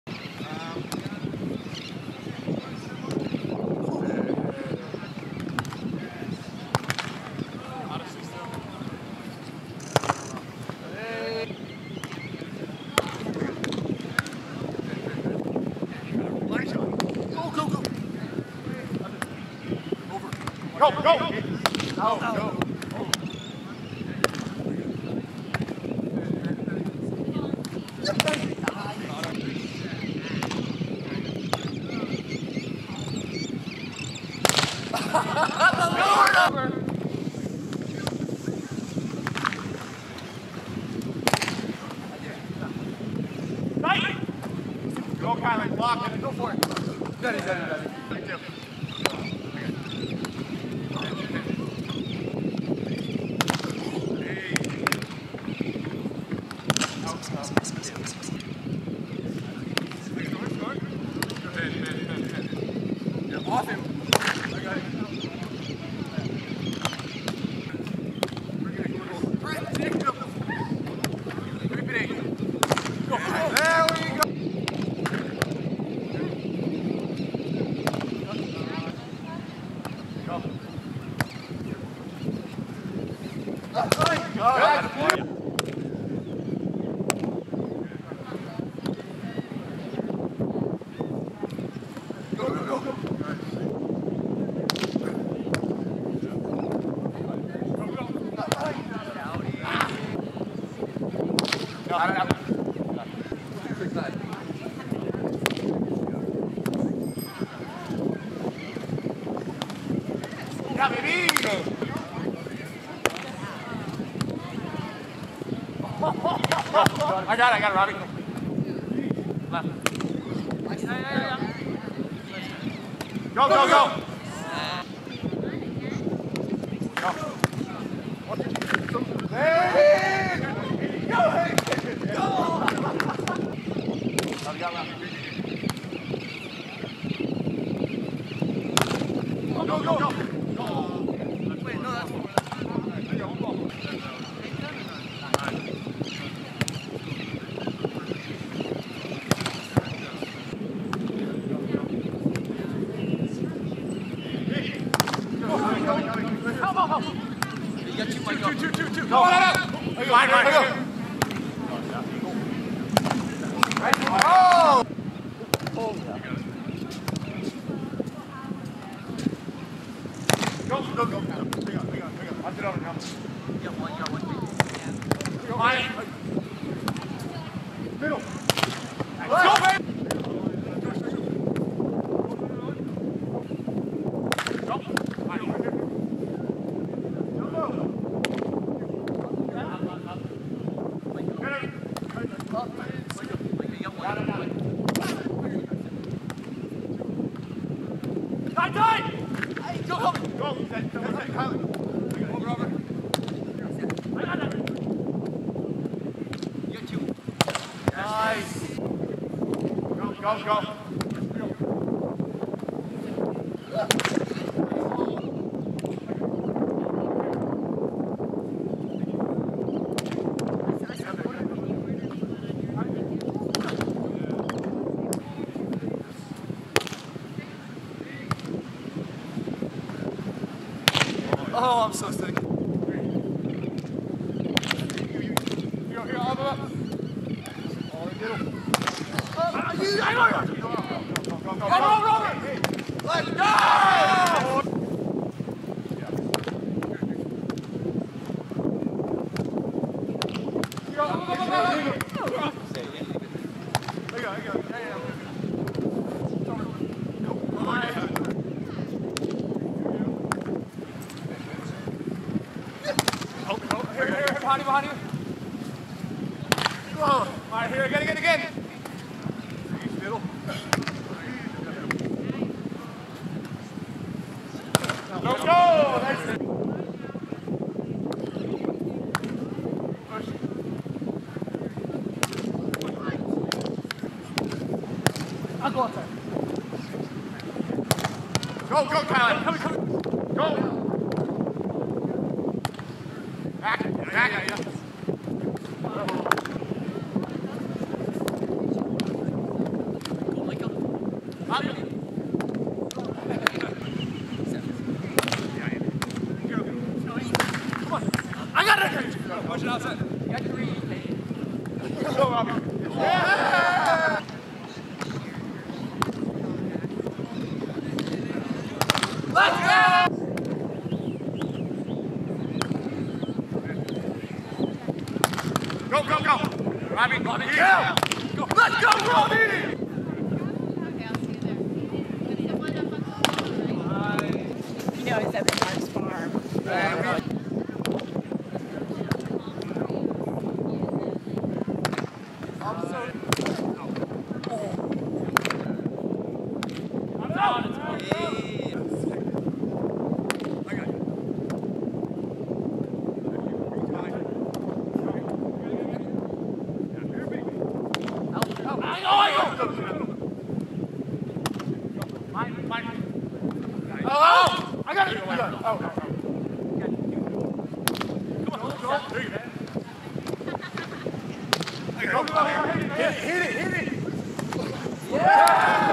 4 Man Beach Crabb/Ma'a/Worsley/Martin vs Paige/Smith/Santa Cruz
Full first 2 sets of the 4 man beach volleyball between Taylor Crabb, Micah Ma'a, Joe Worsley, Oliver Martin vs Robbie Paige, Hagen Smith, and their partners from Santa Cruz. The winner takes home 130 each.